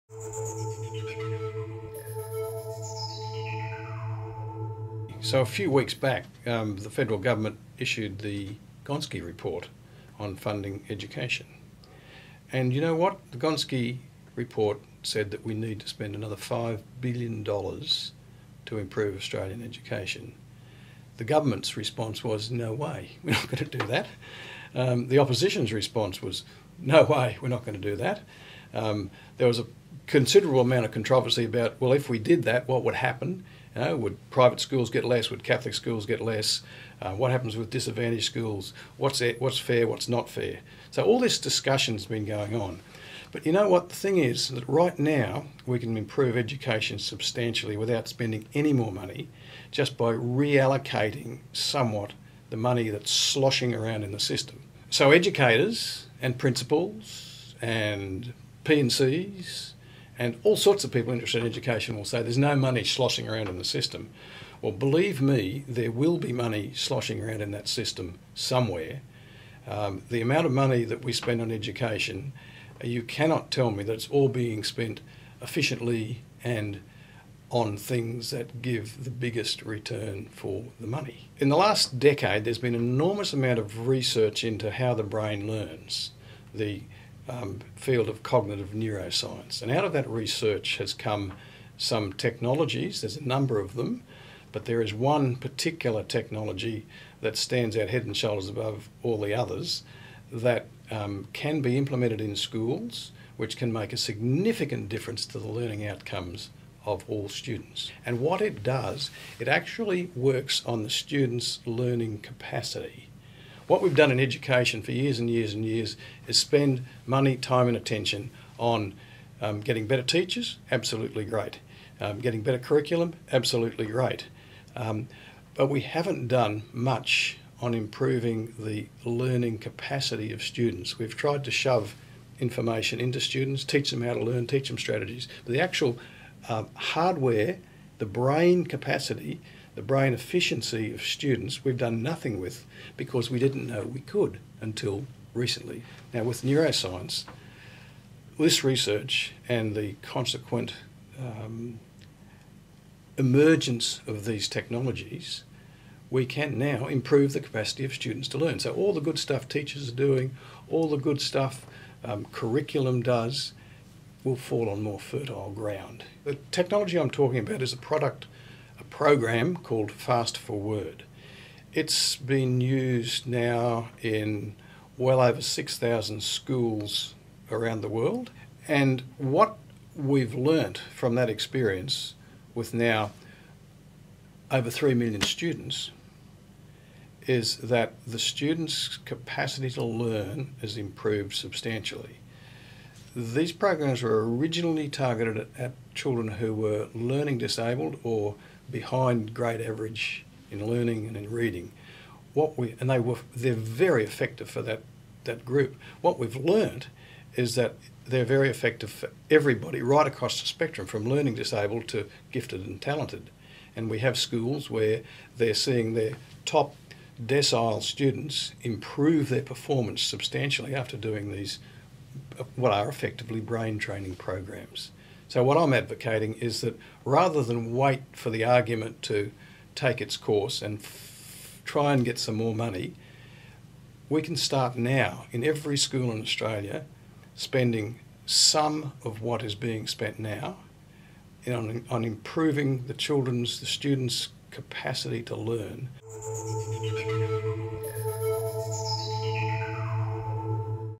Students' Brains - The Key to Better Education
Instead of spending more money on education, as proposed by the Australian government enquiry into education funding (the Gonski Report) and governments all around the world, use neuroscience based technology to boost the capacity of students' brains to assimilate the existing education being delivered by teachers everywhere.